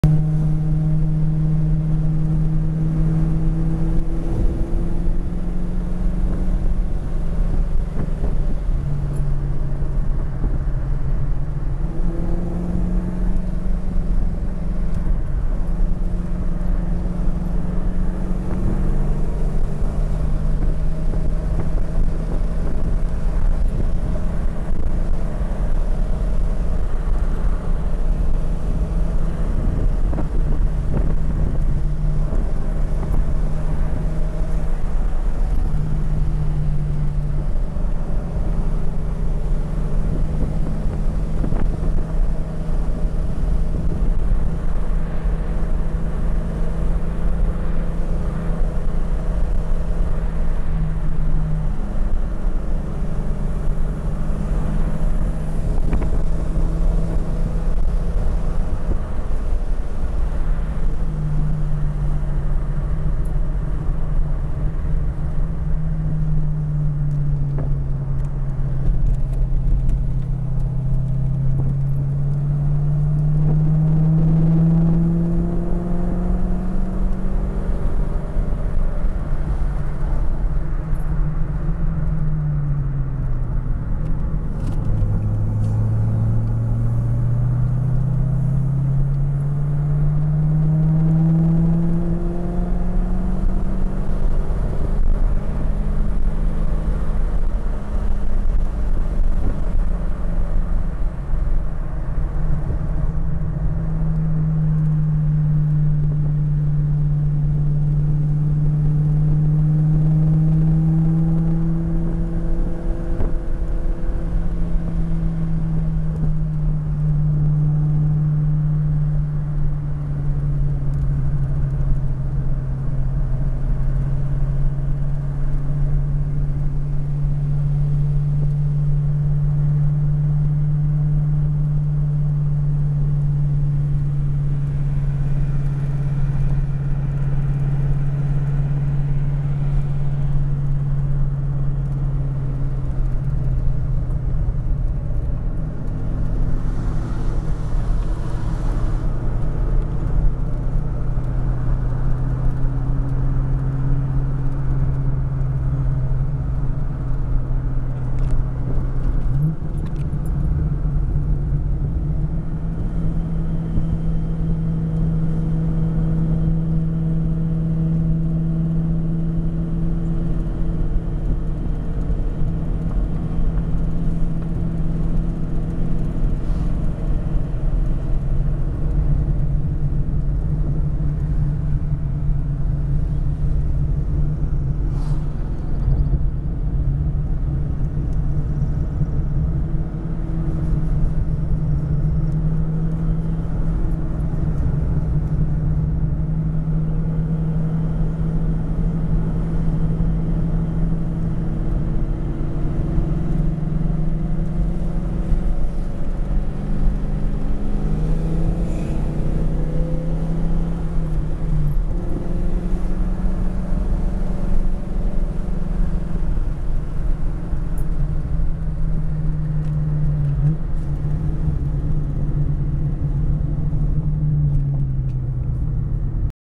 the road up Harris Hill
driving up Harris Hill...while holding the camera in hand. June 20, 2010